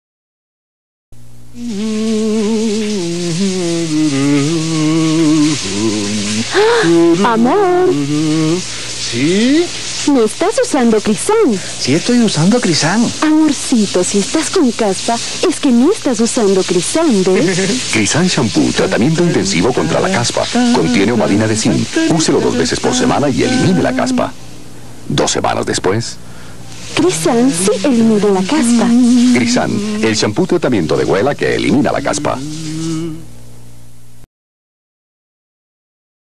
Shampoo Crisán de Wella
Publicidad para televisión de WELLA con su producto: SHAMPOO CRISAN contra la caspa